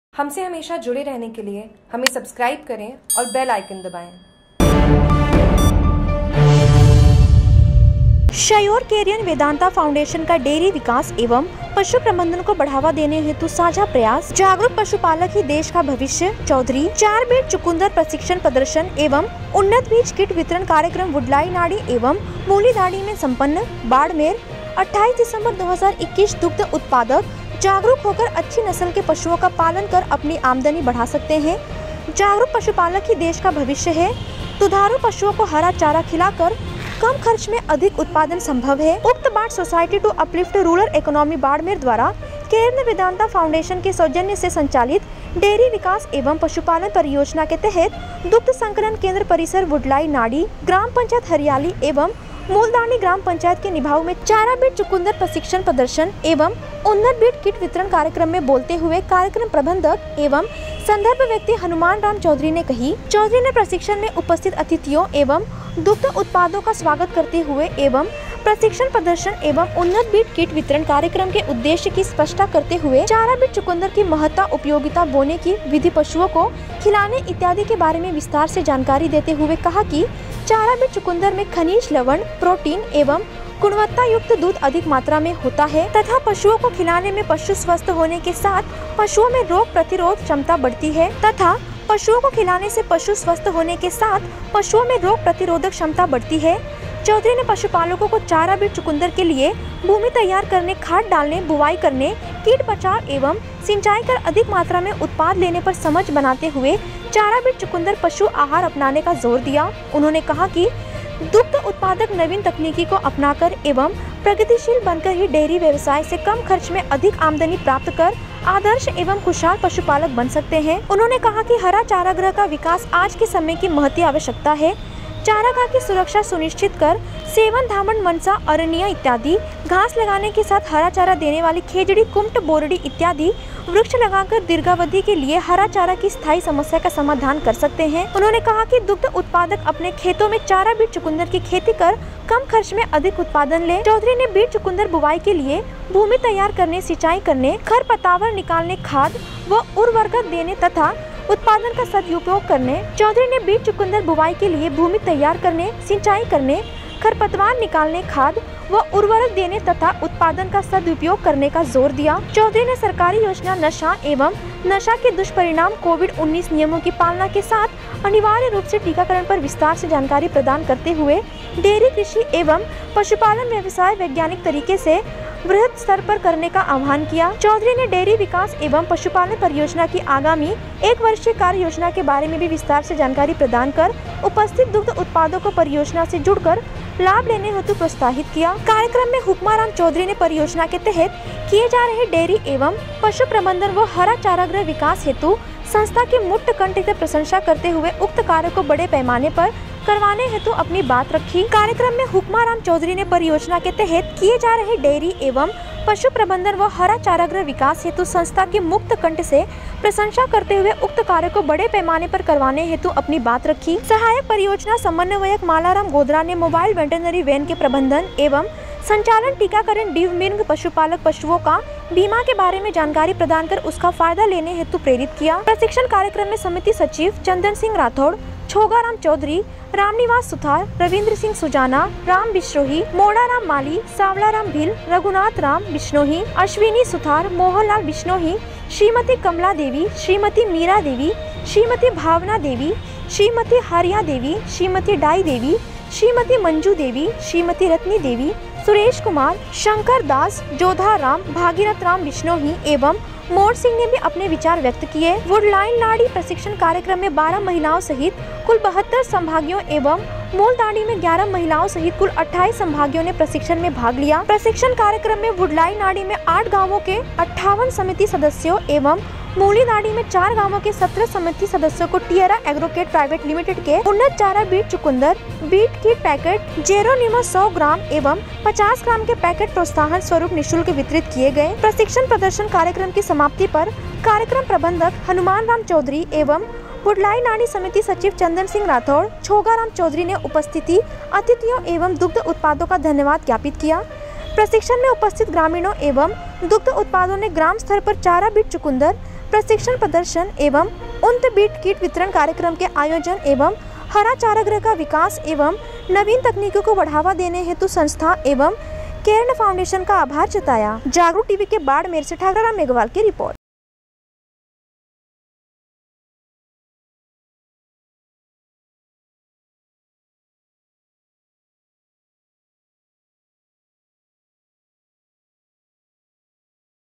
बाड़मेर : जागरूक पशुपालक ही देश का भविष्य- चौधरी
28 दिसंबर 2021 दुग्ध उत्पादक जागरूक होकर अच्छी नस्ल के पशुओं का पालन कर अपनी आमदनी बढ़ा सकते हैं| जागरूक पशुपालक ही देश का भविष्य है| दुधारू पशुओं को हरा चारा खिलाकर कम खर्च में अधिक उत्पादन संभव है | उक्त बात सोसायटी  टू अपलिफ्ट रूरल  इकोनामी बाड़मेर द्वारा केयर्न वेदांता फाउंडेशन के सौजन्य से संचालित डेयरी विकास एवं पशुपालन परियोजना के  तहत दुग्ध संकलन केंद्र परिसर बुड्ढलाई नाड़ी  ग्राम पंचायत  हरियाली एवं मूली ढाणी  ग्राम पंचायत निभाउ मैं चारा बिट चुकंदर प्रशिक्षण प्रदर्शन एवं उन्नत बीज किट वितरण  कार्यक्रम मैं बोलते हुए  कार्यक्रम प्रबंधक  एवं संदर्भ व्यक्ति हनुमान राम चौधरी ने कहीं |चौधरी ने प्रशिक्षण मैं उपस्थित अतिथियों एवं दुग्ध उत्पादकों का स्वागत करते हुए एवं प्रशिक्षण  प्रदर्शन एवं उन्नत बीज किट वितरण  कार्यक्रम के उद्देश्य की स्पष्टता करते हुए चारा बिट चुकंदर की महता उपयोगिता बोने की विधि पशुओं को  खिलाने इत्यादि के बारे में विस्तार से जानकारी देते हुए कहा कि चारा बीट चुकंदर में खनिज लवण प्रोटीन एवं गुणवत्ता युक्त दूध अधिक मात्रा में होता है| तथा पशुओं को खिलाने से पशु स्वस्थ होने के साथ पशुओं में रोग प्रतिरोधक क्षमता बढ़ती है| चौधरी ने पशुपालकों को चारा बीट
 चुकंदर के लिए भूमि तैयार करने खाद डालने बुवाई करने कीट बचाव एवं सिंचाई कर अधिक मात्रा में उत्पाद लेने पर समझ बनाते हुए चारा बीट चुकंदर पशु आहार अपनाने पर जोर दिया |उन्होंने कहा कि दुग्ध उत्पादक नवीन तकनीकी को अपनाकर एवं प्रगतिशील बनकर ही
  डेयरी व्यवसाय से कम खर्च में अधिक आमदनी प्राप्त कर आदर्श एवं खुशहाल पशुपालक बन सकते हैं | उन्होंने कहा कि हरा चारागाह का विकास आज के समय की महती आवश्यकता है | चारागाह की सुरक्षा सुनिश्चित कर सेवन धामण मनसा अरनिया इत्यादि घास लगाने के साथ हरा चारा देने वाली खेजड़ी कुम्मट बोरडी इत्यादि वृक्ष लगाकर दीर्घावधि  के लिए हरा चारा की स्थाई समस्या का समाधान कर सकते हैं |उन्होंने कहा कि दुग्ध उत्पादक अपने खेतों में चारा बीट चुकंदर की खेती कर कम खर्च में अधिक उत्पादन ले |चौधरी ने बीट चुकंदर  बुवाई के लिए भूमि तैयार करने  सिंचाई करने खरपतवार निकालने खाद व उर्वरक देने  तथा उत्पादन का सदुपयोग करने पर जोर दिया  |चौधरी ने सरकारी योजना  नशा एवं नशा के दुष्परिणाम  कोविड-19 नियमों की पालना  के साथ  अनिवार्य रूप से  टीकाकरण पर विस्तार से  जानकारी  प्रदान करते हुए डेयरी  कृषि एवं पशुपालन व्यवसाय  वैज्ञानिक तरीके से वृहत स्तर पर करने का आह्वान किया| चौधरी ने  डेयरी विकास  एवं पशुपालन परियोजना की  आगामी  एक वर्षीय  कार्य योजना के बारे में भी  विस्तार से जानकारी  प्रदान कर  उपस्थित दुग्ध उत्पादकों को  परियोजना से जुड़कर  लाभ लेने हेतु  प्रोत्साहित किया| कार्यक्रम मे हुकमा राम चौधरीने परियोजना के तहत  किए जा रहे डेयरी एवं पशु प्रबंधन व हरा चारागाह विकास हेतु संस्था की मुक्त कंठ से प्रशंसा करते हुए उक्त कार्य को बड़े पैमाने पर करवाने हेतु अपनी बात रखी|